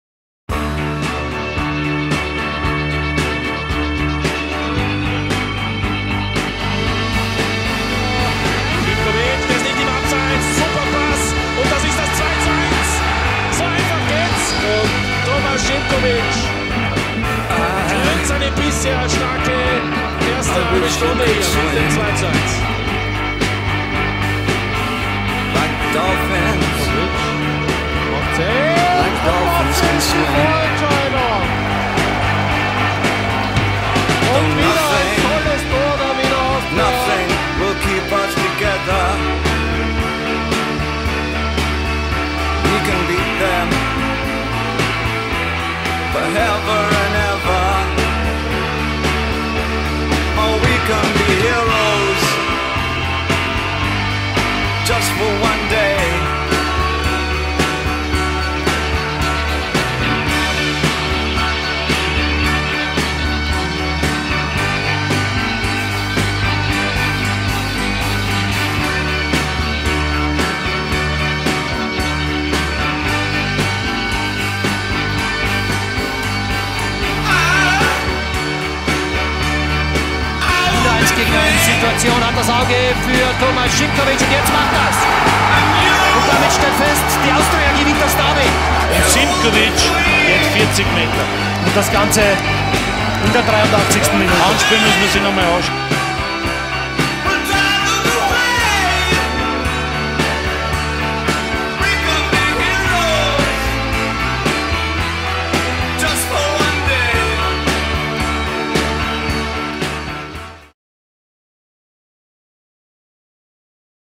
Tomas Simkovic - Meister 2013
Einige Assists und Tore aus der Meistersaison mit Austria Wien.